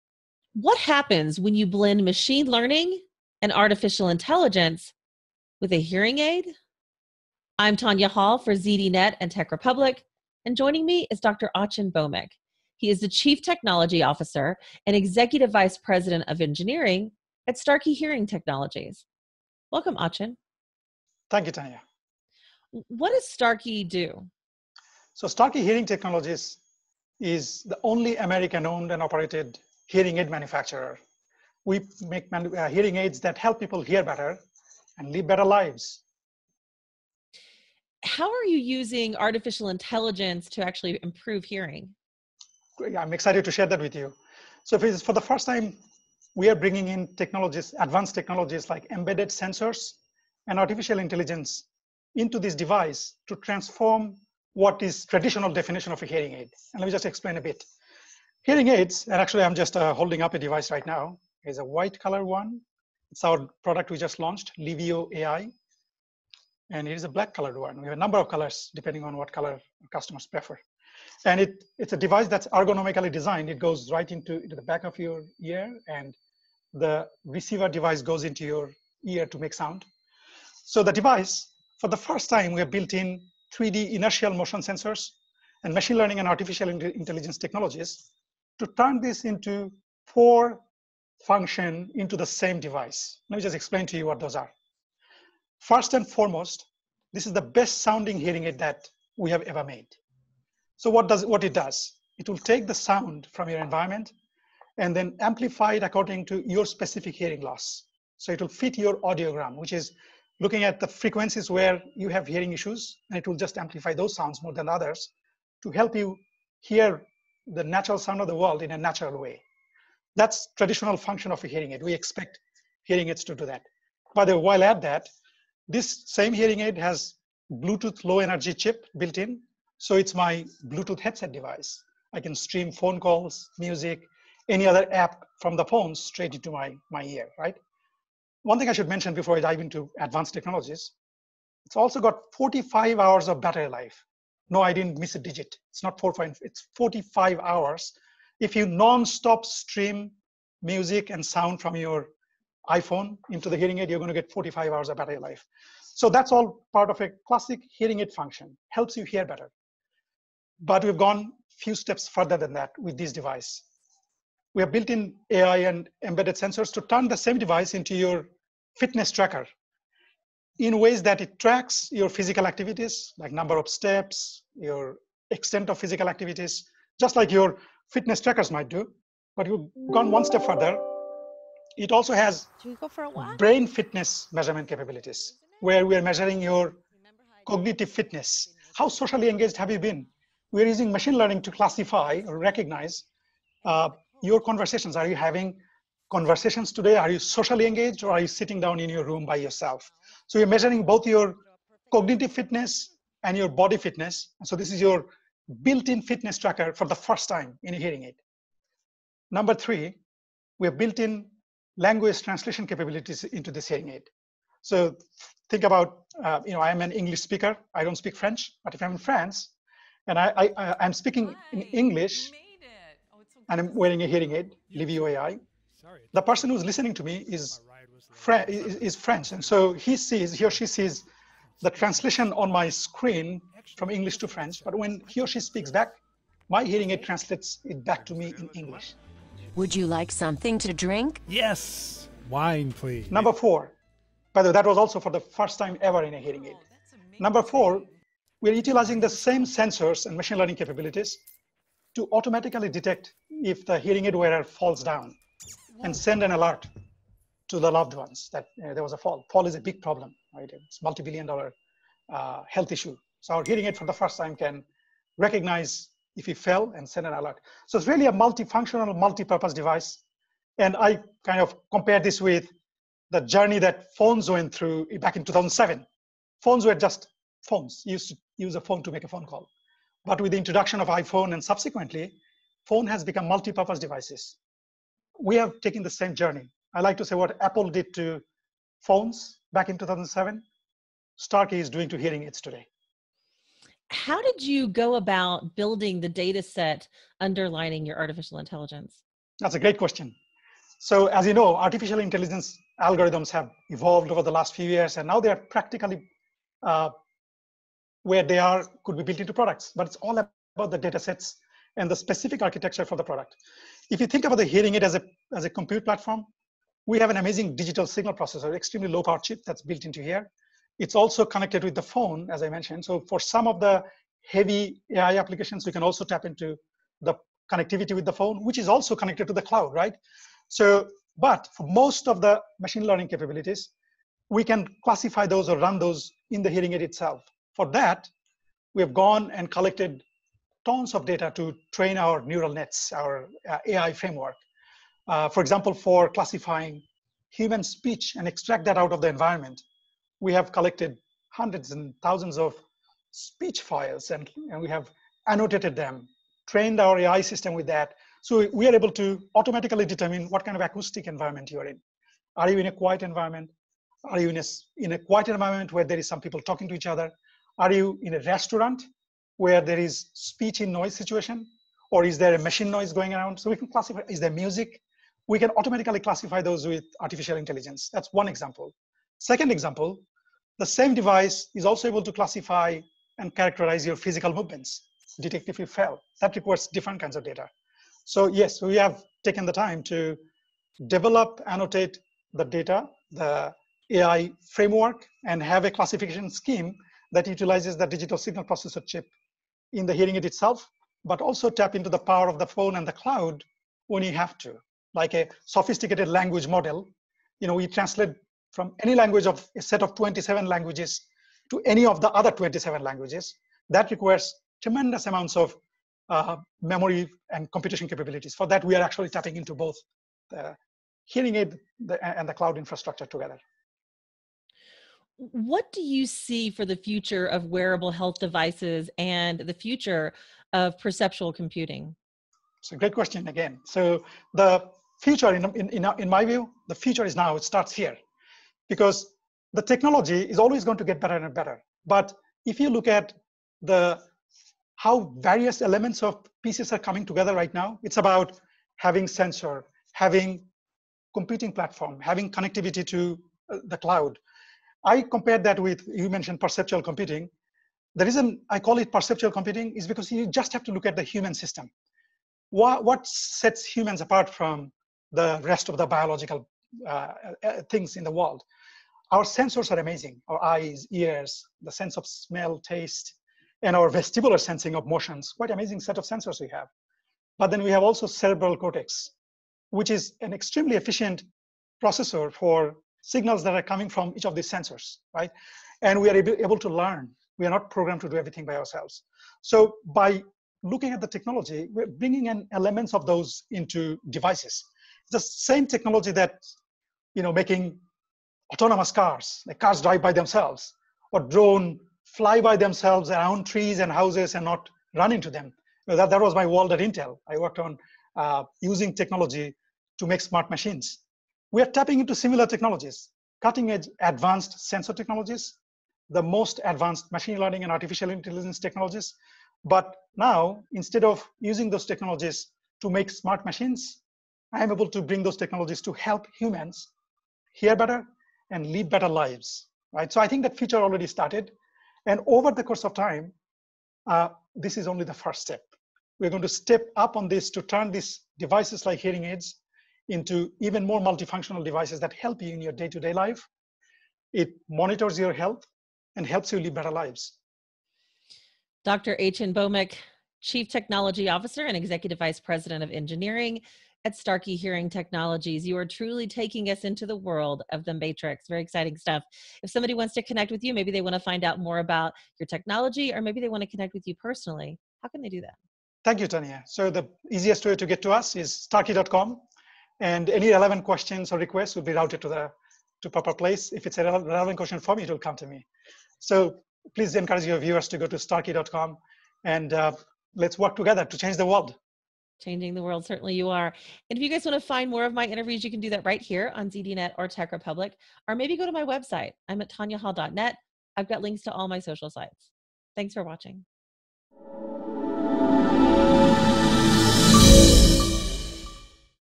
Adding AI to hearing aids | ZDNet
Tonya Hall and Dr. Achin Bhowmik, chief technology officer and executive vice president of engineering at Starkey Hearing Technologies, discuss the future of wearable and perceptual computing.